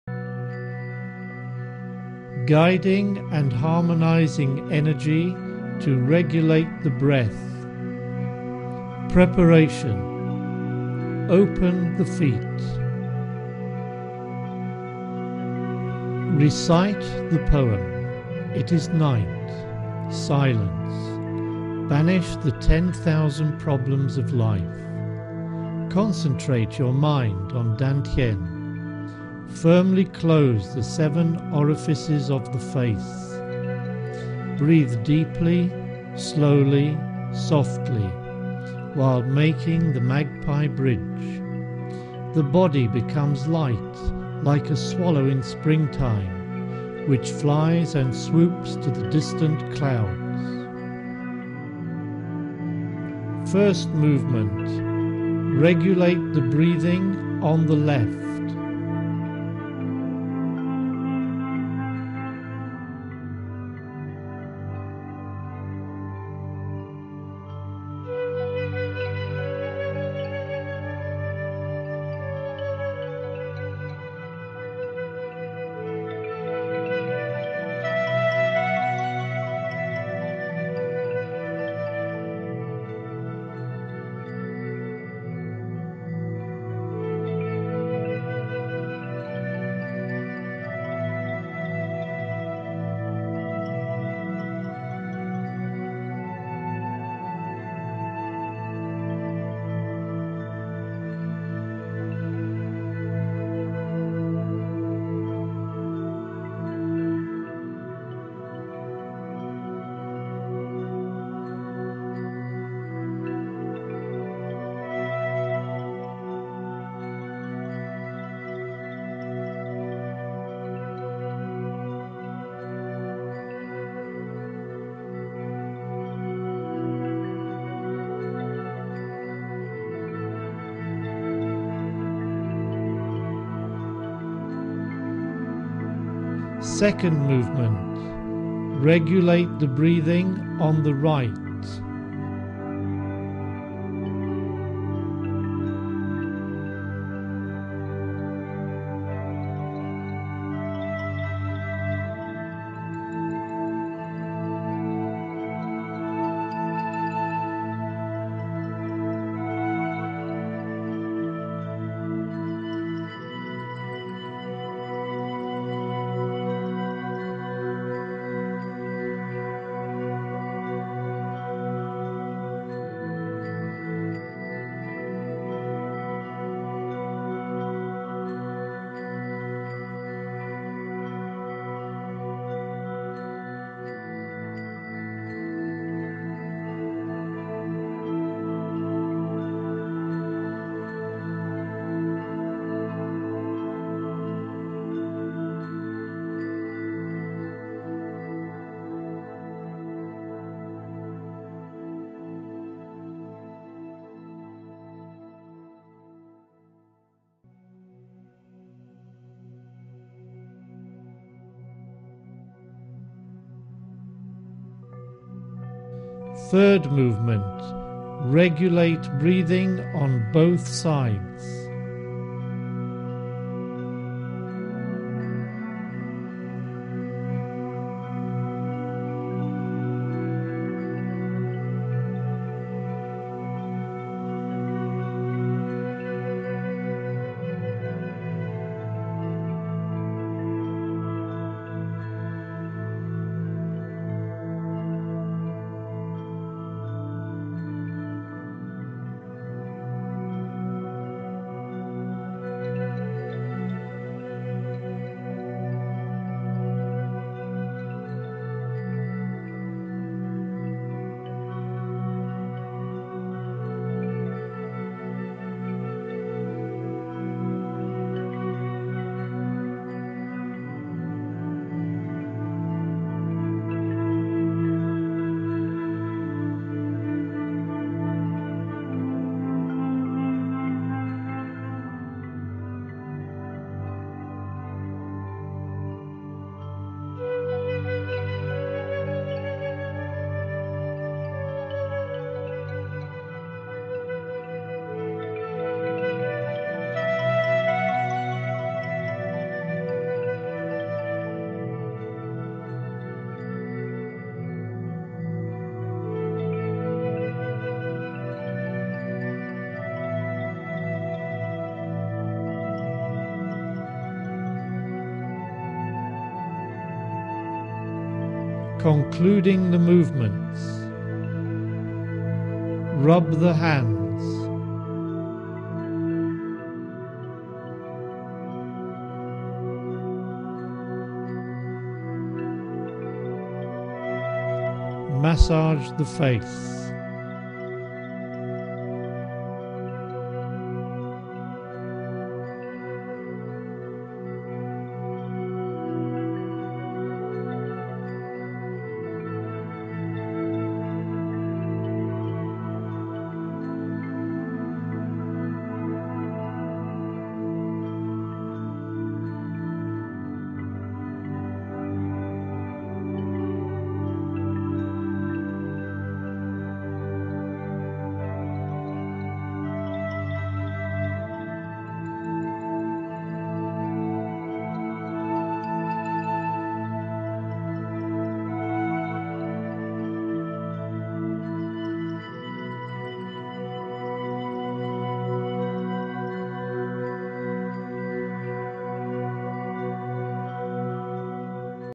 Dao yin yang sheng gong - regulate the breath sequence
A great qigong or chi gong sequence from professor Zhang Guangde: Guiding and Harmonising Energy to Regulate the breath. It's best to read the book to understand this sequence and get the most out of it.

If you want to know which ones
But be aware that the books assume you know the basics about Qi and certain "points" like for example the Dan Tian and Ming Men... But a little research on google certainly helps :)